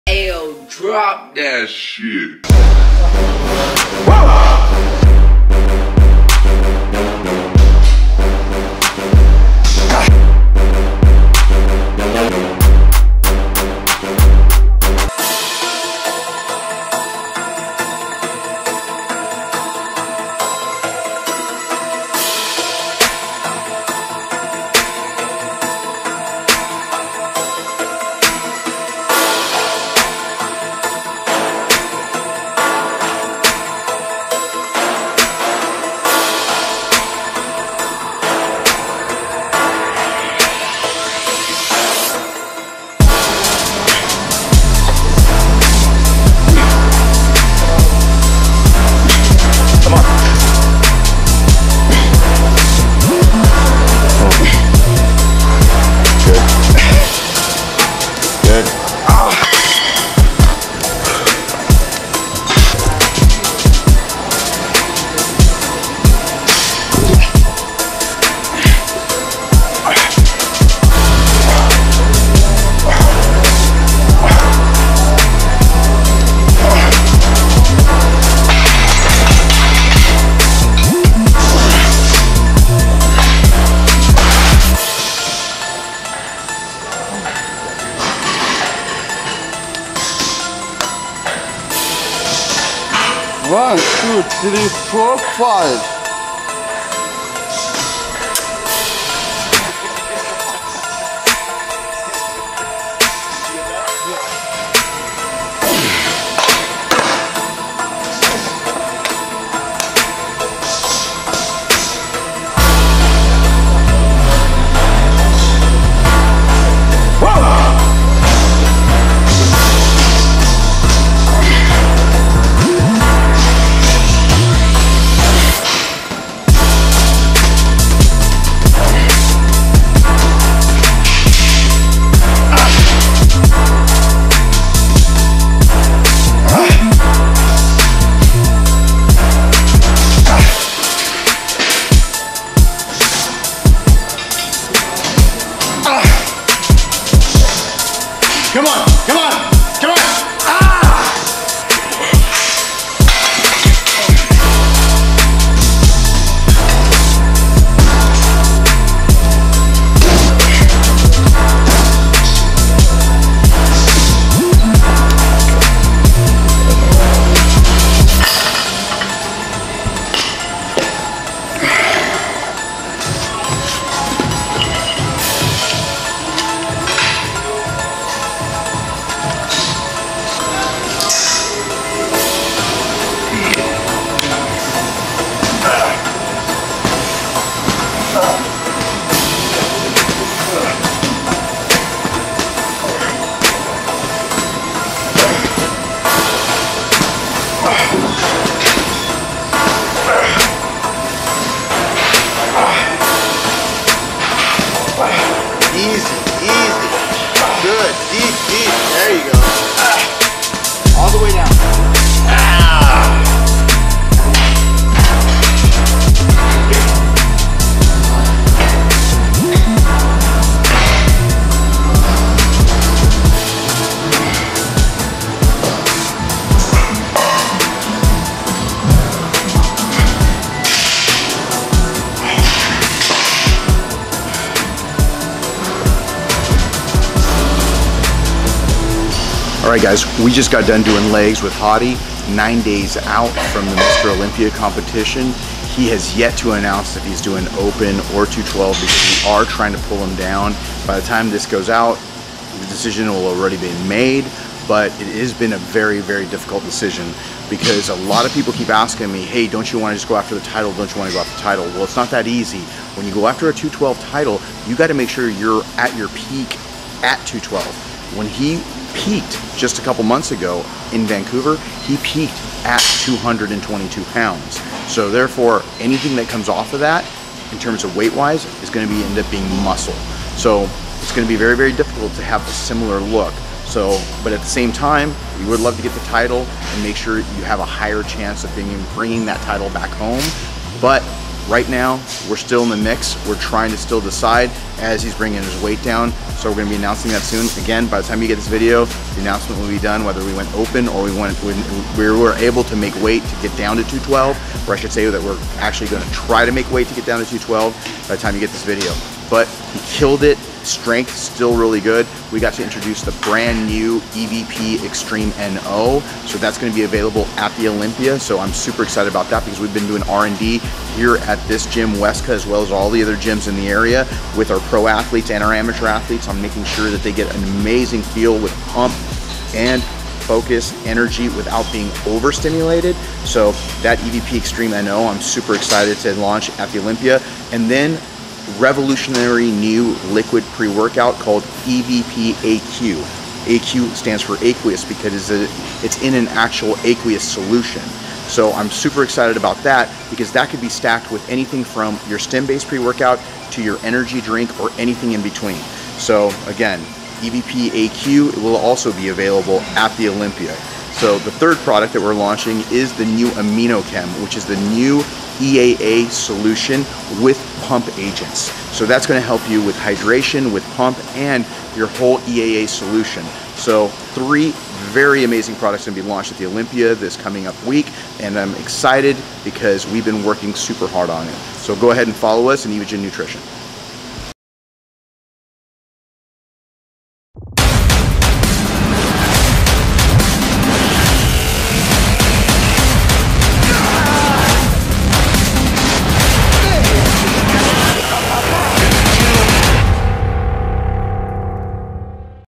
Train with The Pro Creator: Hadi Choopan FST-7 Quads One Week Out
19X Olympia winning coach Hany Rambod gets IFBB Pro Hadi Choopan dialed in for his Olympia debut. Follow The Pro Creator as he puts the finishing details on Hadi's legs at WESTCA in San Jose a week out from the Mr. Olympia. The Iranian bodybuilder is set to live his dream to compete on the world's biggest stage in Las Vegas.